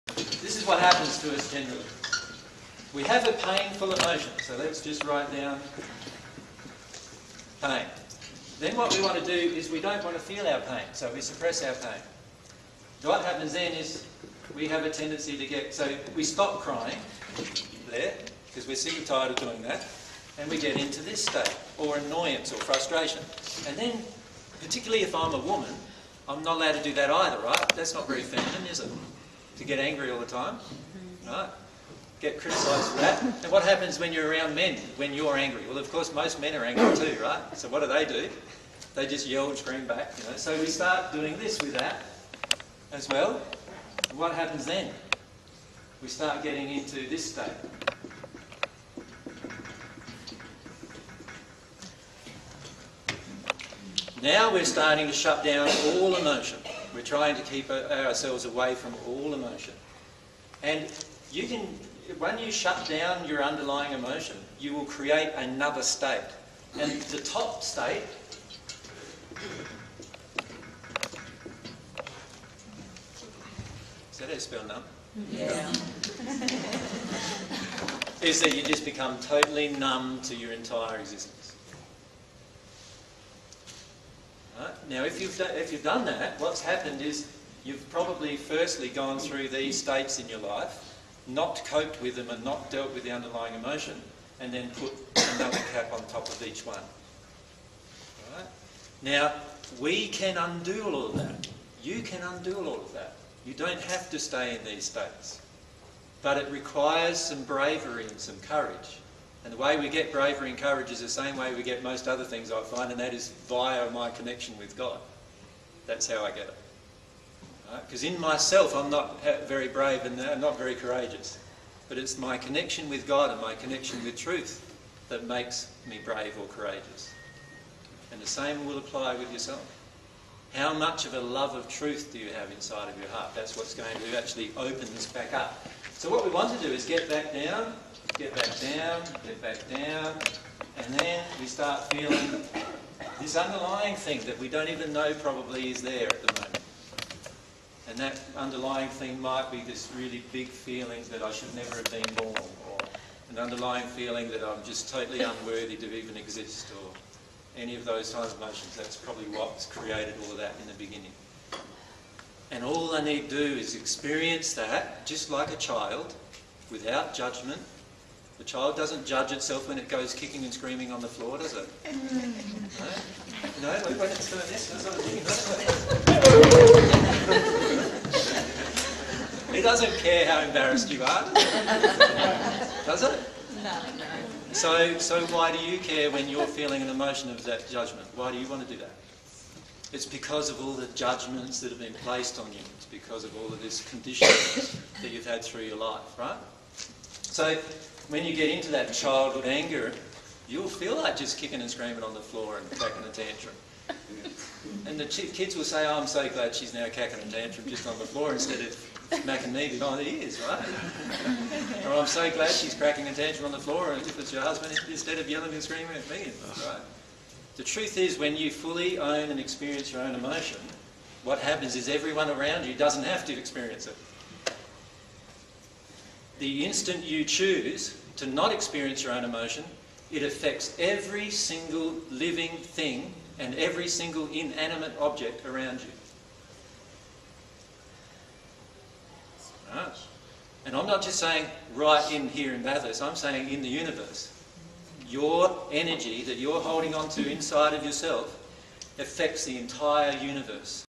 Jesus on How Shutting Down Our Emotions Impacts the Universe
Jesus describes how our choice to ignore and shut down our feelings impacts the universe.

This is a section of the presentation by Jesus entitled "20080914 General Discussion - Q&A From People In Bathurst P1".

dtstart:004649:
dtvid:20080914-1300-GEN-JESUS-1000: